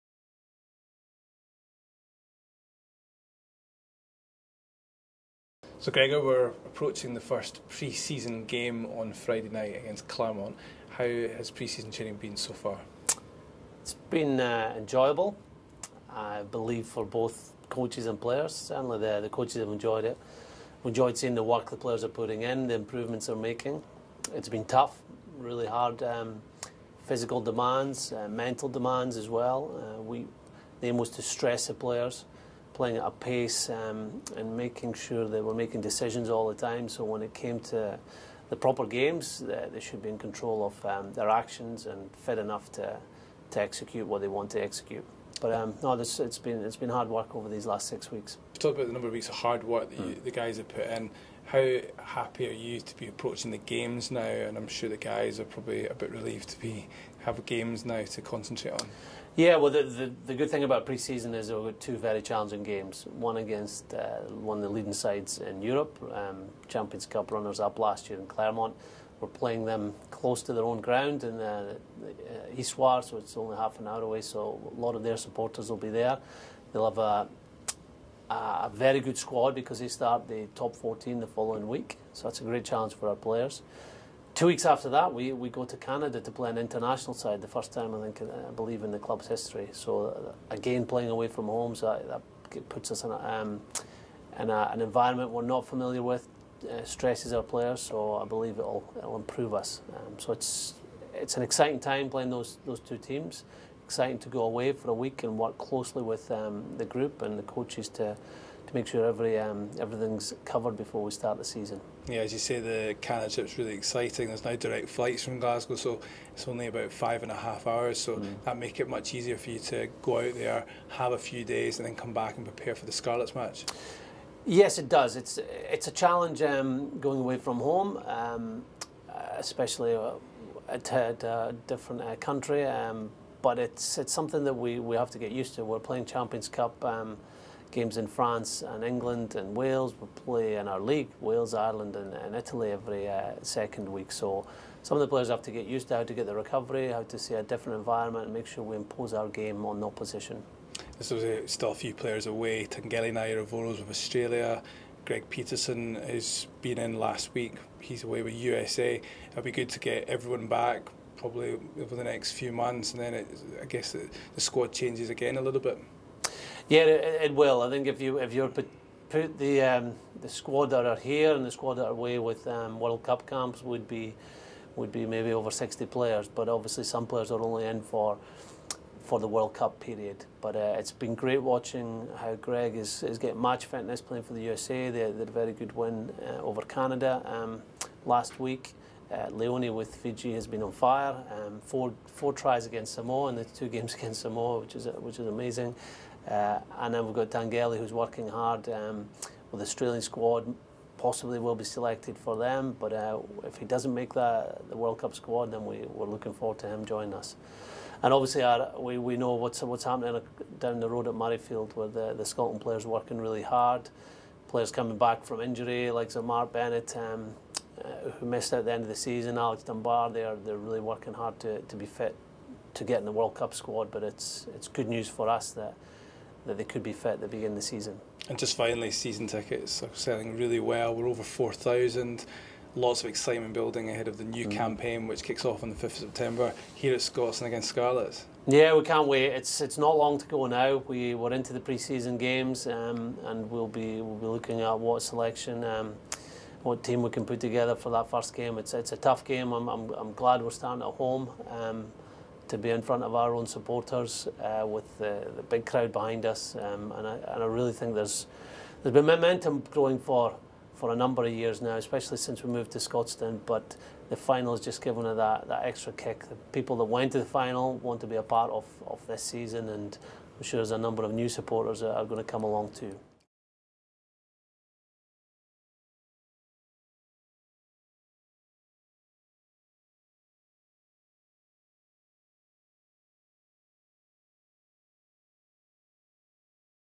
Exclusive Gregor Townsend Interview
Gregor Townsend speaks to GWTV about the upcoming pre-season games against Clermont & Canada and looks ahead to the new season